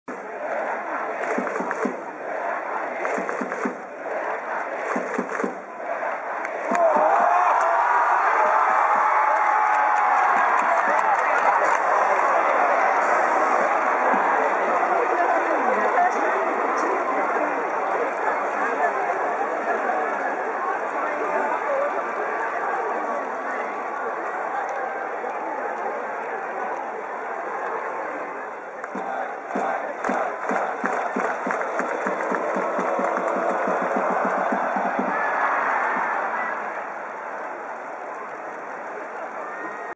ヤクルト 村上宗隆 あわやホームランの大ファール ヤクルト vs 広島 2024年9月19日(木) 神宮球場
2024年9月19日(木)　ヤクルト vs 広島　神宮球場　現地で撮影
5回裏、村上宗隆がライトスタンドに飛距離充分の大ファール。その後、ハーフスイングで空振り三振。